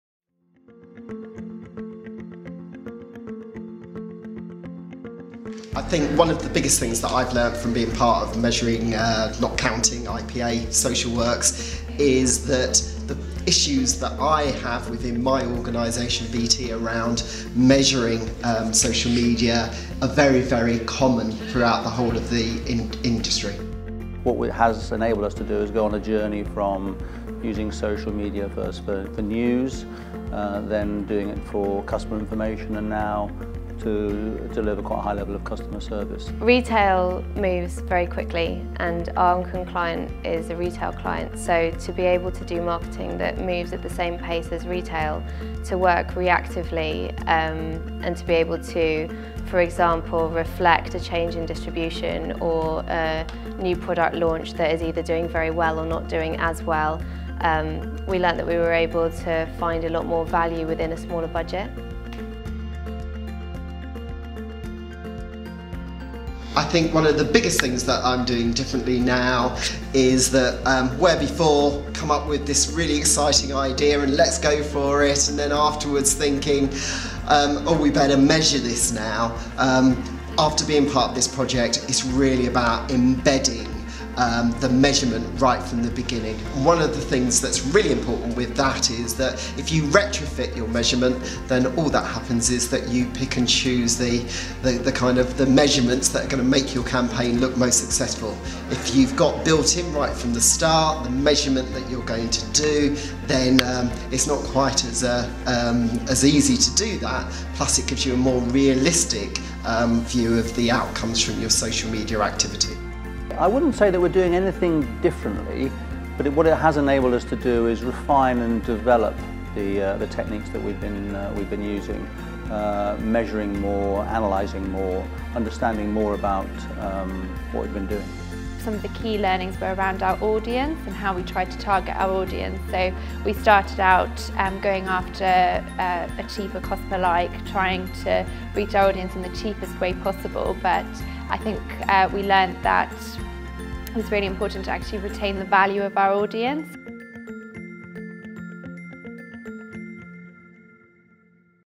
IPASocialWorks Guide Launch: Case Study Interviews
Group members and case study contributors such as Christopher Wellbelove, Chris Macleod, Elle Graham-Dixon and Cairen Harry reveal their thoughts at the guide launch of the cross-industry IPASocialWorks social media measurement and effectiveness project at the IPA's headquarters in London on 2nd February 2015. or learn more about how to get involved